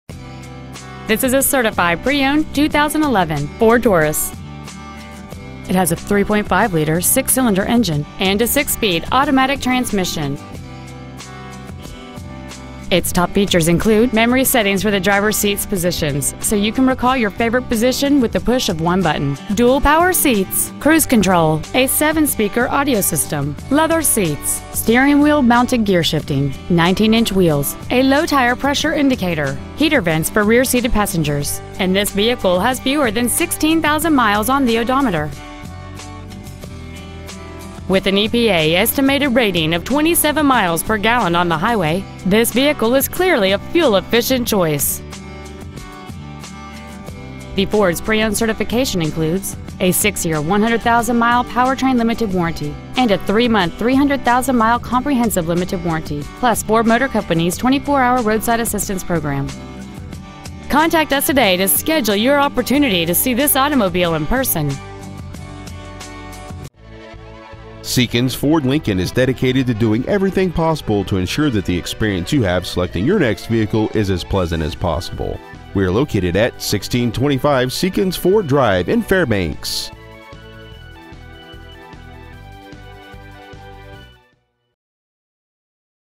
2011 FORD TAURUS Certified AK
We have been honored to serve the  AK area , we promise that your experience at our dealership will exceed your expectations !

 Year : 2011
 Make : FORD
 Model : TAURUS
 Engine : 3.5L V6
 Trans . : AUTO 6SPD
 Exterior : BLUE
 Miles : 15,519

 Stock : CP4190X

 1625 Seekins Ford Drive
  , AK 99701